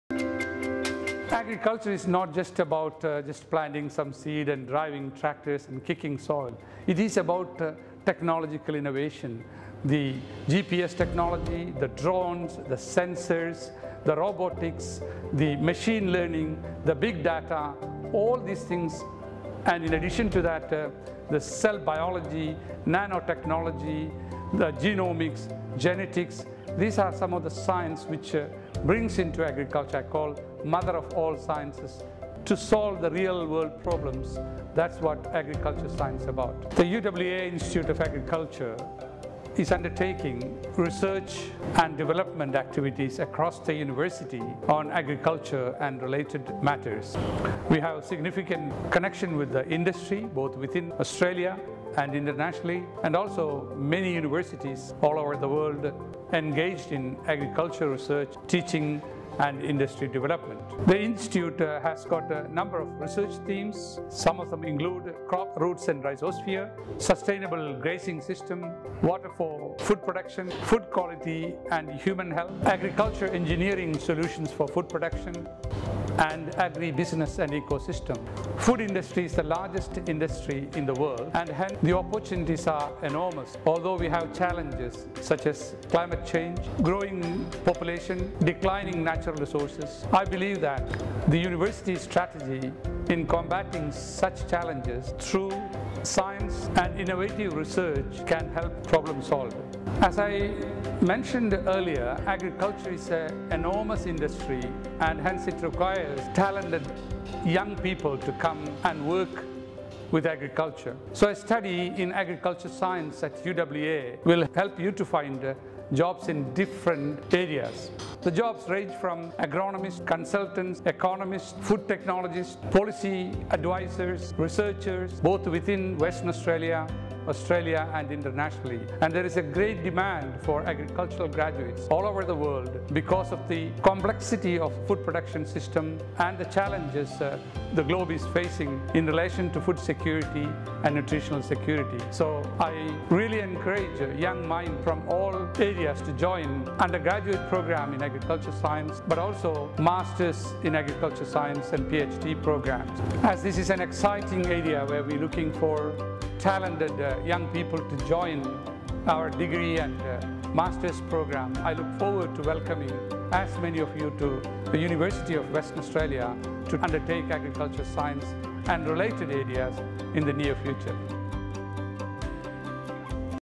Why agriculture is the "mother of all sciences" according to UWA IOA Director Kadambot Siddique
The University of Western Australia's Institute of Agriculture Director Kadambot Siddique describes how we can face the world's greatest challenges through innovative agricultural science research.

"Agriculture is not just about just planting some seed, driving tractors and kicking soil," Hackett Professor Siddique says. "It is about technological innovation and combating challenges such as climate change, growing populations and declining resources."